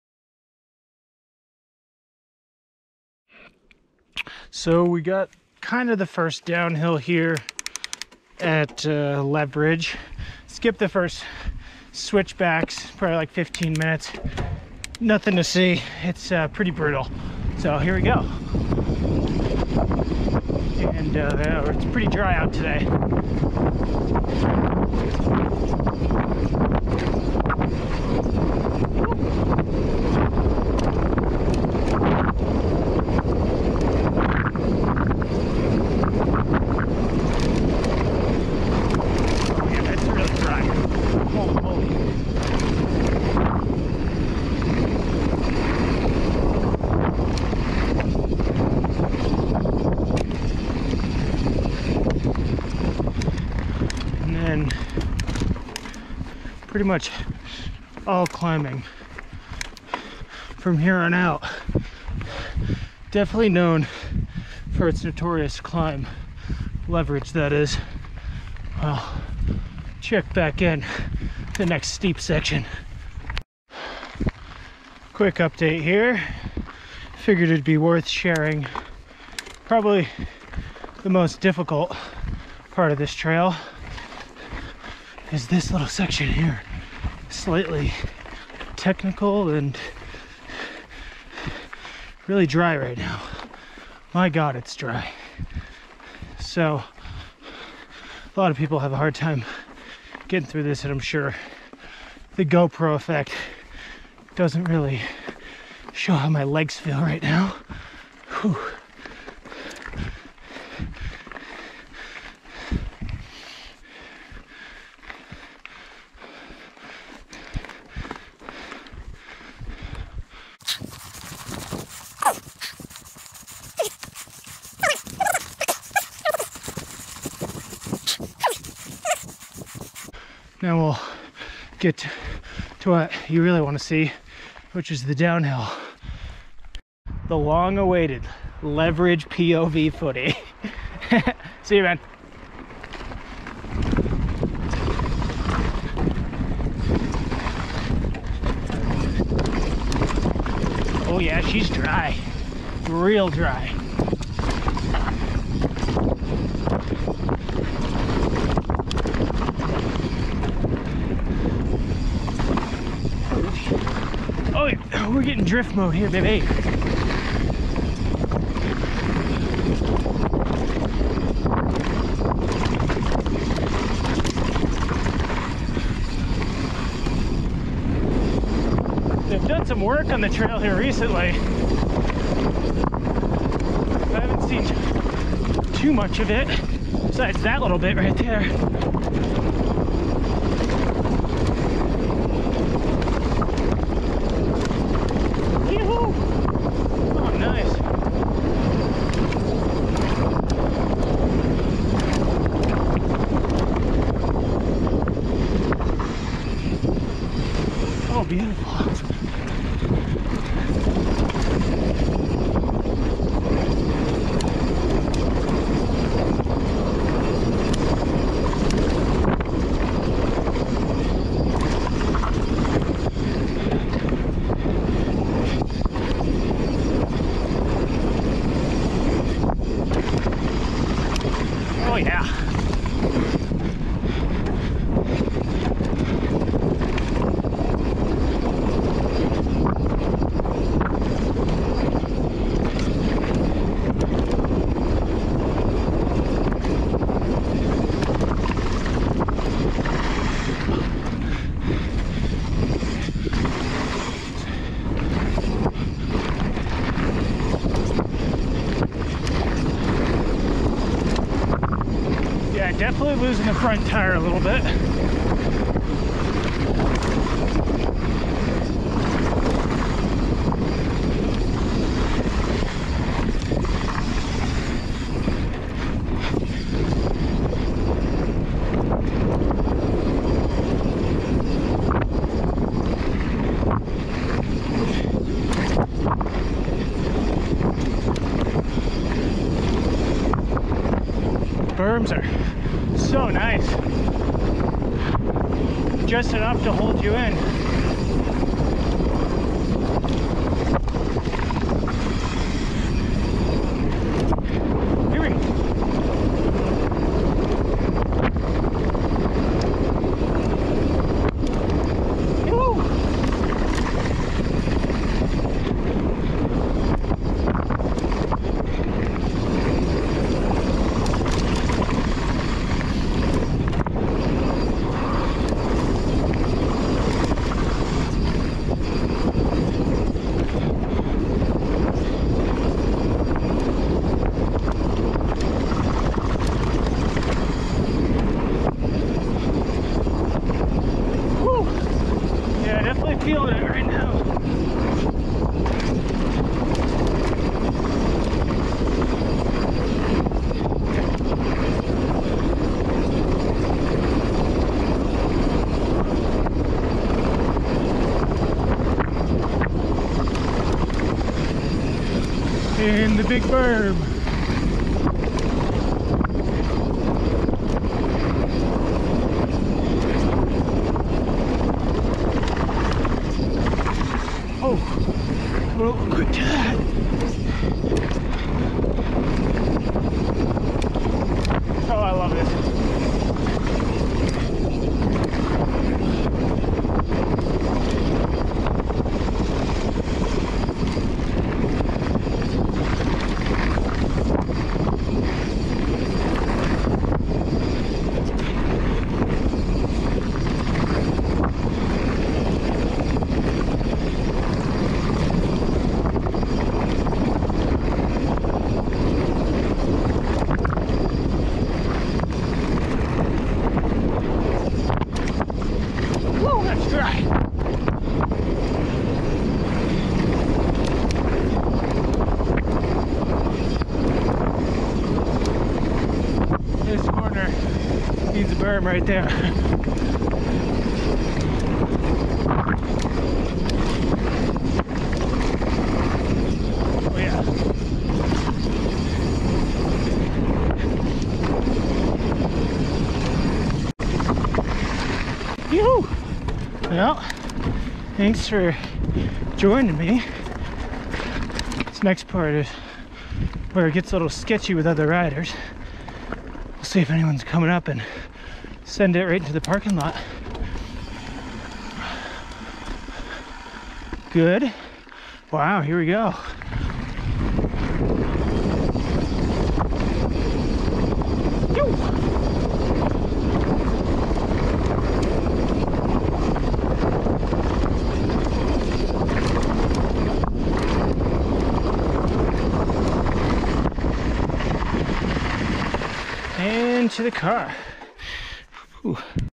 Leverich Canyon Shred Sesh!
As you may have heard, it's a bit dry here in Montana. More biking in the rocky mountains to come.